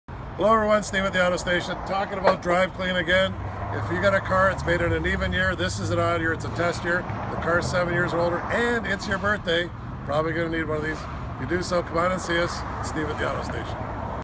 Is it your Birthday in September? Then your car's Drive Clean Inspection should be scheduled soon!
If your Birthday is coming up in September, your likely will need to be Drive Clean tested.